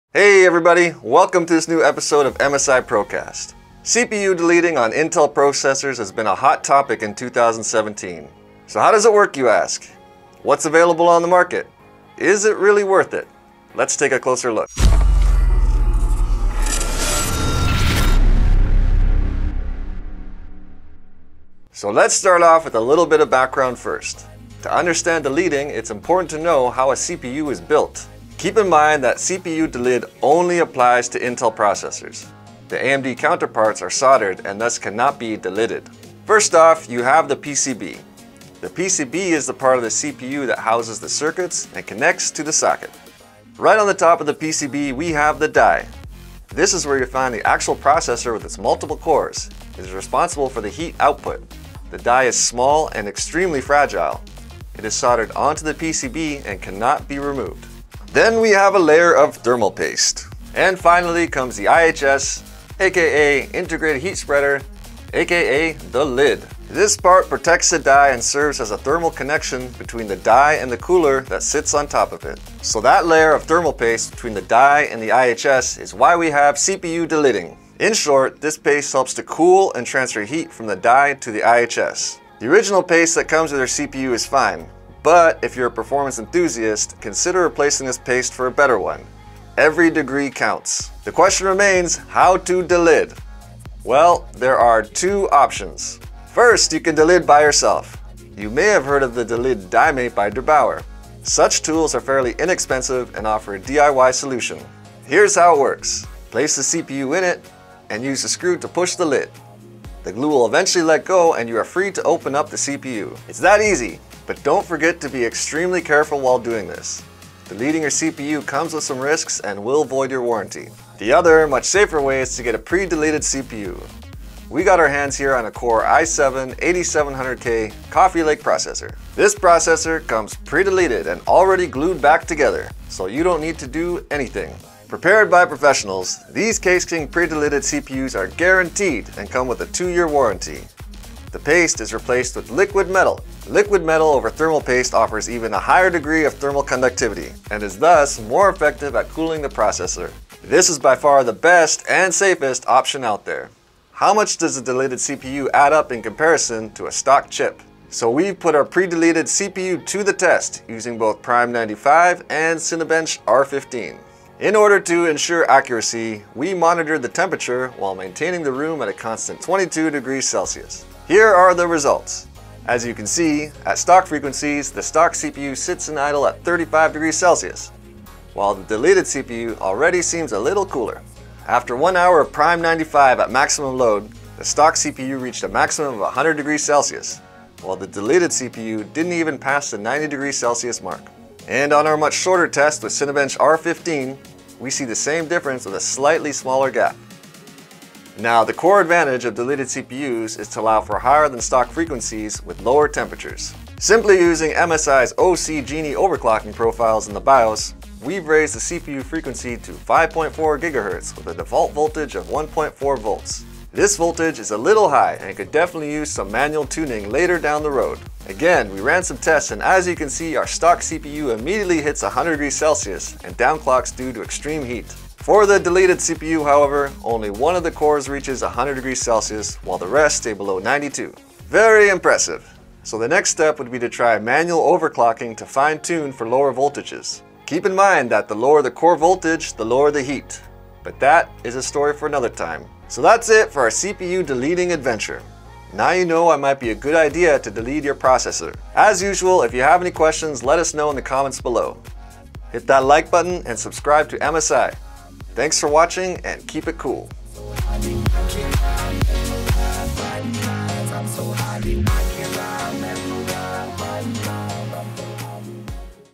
MSI Pro Cast#18 –How to delid an Intel Core i7 8700K CPU? | Gaming Motherboard
CPU delidding on Intel processors has been a hot topic in 2017. in this video we’re going to take a closer look at the technique. How does it work you ask? And, what’s available on the market? Is it really worth it? What's the difference between delidded CPU and stock CPU? Let’s take a closer look.